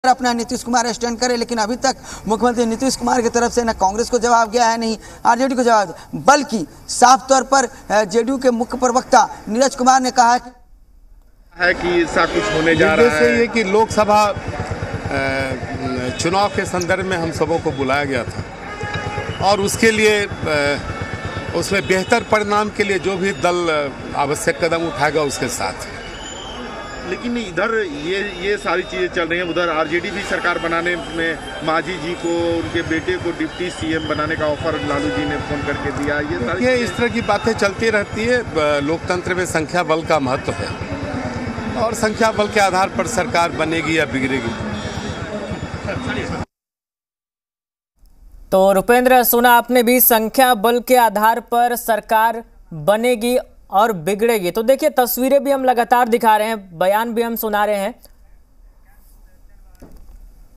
Bihar Political Crisis : बिहार के हालात पर Tarkishore Prasad की प्रतिक्रिया | Nitish Kumar
Bihar Political Crisis : बिहार में सियासी तूफान जारी है...हर सियासी दल अपनी-अपनी रणनीति बना रही है...एक ओर ताबड़तोड़ बैठकों का दौर चल रहा है...इसी बीच पूर्व उपमुख्यमंत्री तारकिशोर प्रसाद ने बयान दिया है...'बेहतर परिणाम के लिए जो भी पार्टी आवश्यक कदन उठाएगा उसके साथ है'